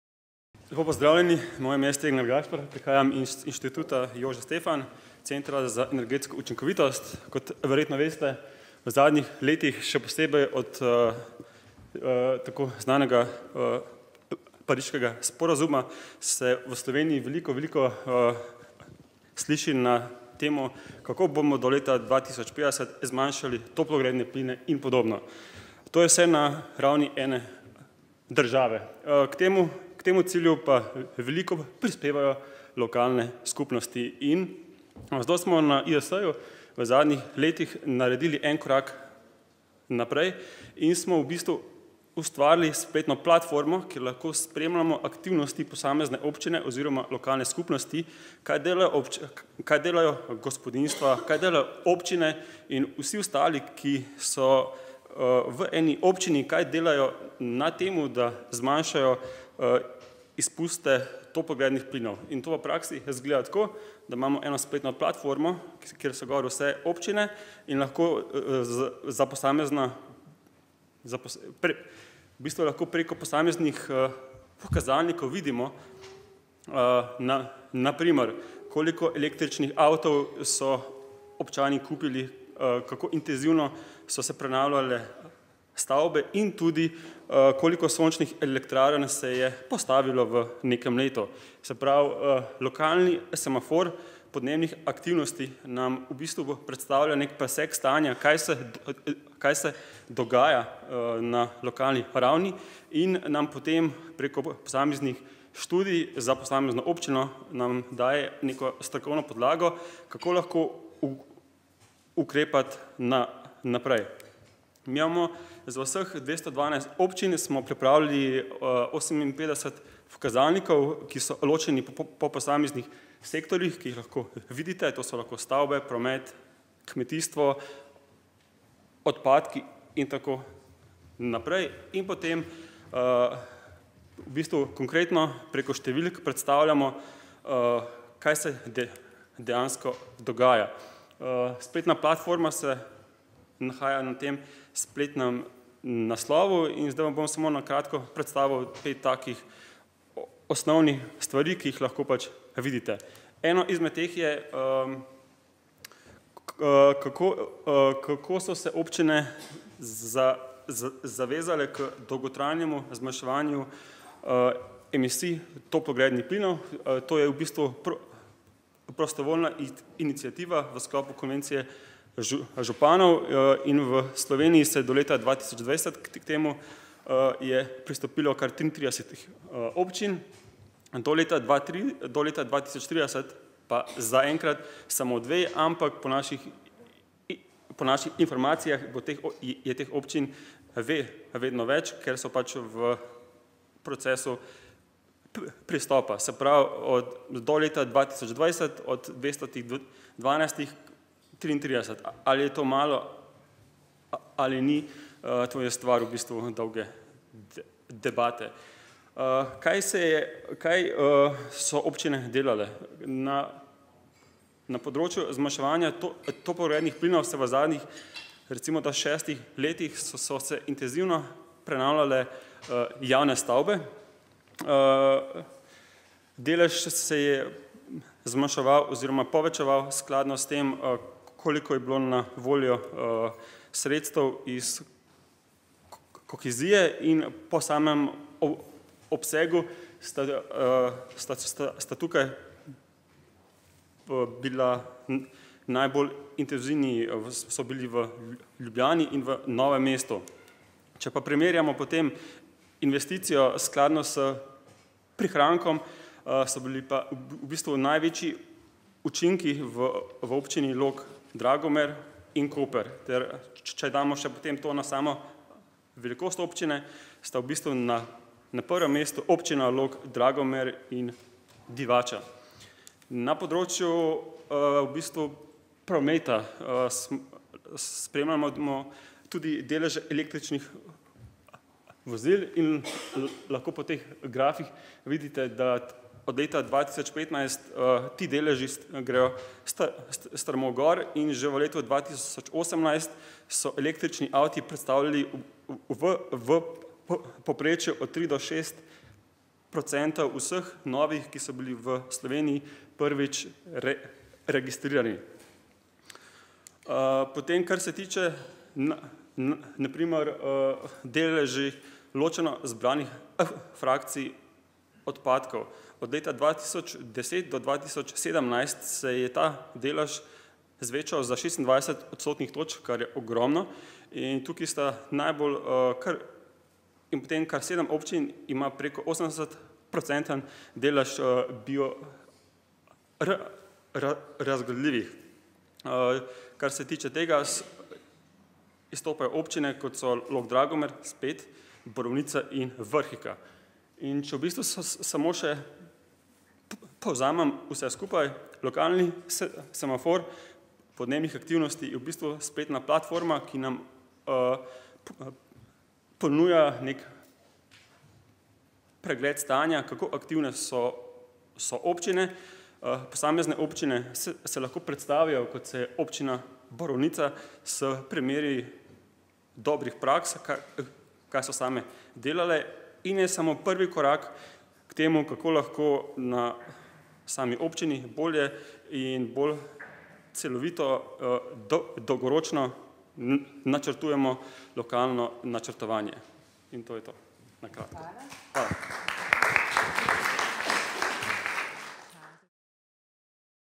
Gašper Stegnar, Institut »Jožef Stefan« – Center za energetsko učinkovitost
4. nacionalne konference o trajnostnem upravljanju z viri skupnosti, ki sta jo organizirala Umanotera in društvo Focus v okviru projekta LIFE CARE4CLIMATE, se je udeležilo več kot 80 predstavnikov občin, razvojnih agencij, civilnodružbenih organizacij in civilnih iniciativ, izobraževalnih ustanov, podjetij in državne uprave, ki so s svojo prisotnostjo potrdili prepoznavnost ključne vloge lokalnih skupnosti v prehodu v nizkoogljično družbo v Sloveniji.

Gašper Stegnar iz Instituta »Jožef Stefan« – Centra za energetsko učinkovitost, je predstavil spletno platformo Lokalni semafor podnebnih aktivnosti, ki prikazuje aktivnosti slovenskih občin na področju blaženja podnebnih sprememb.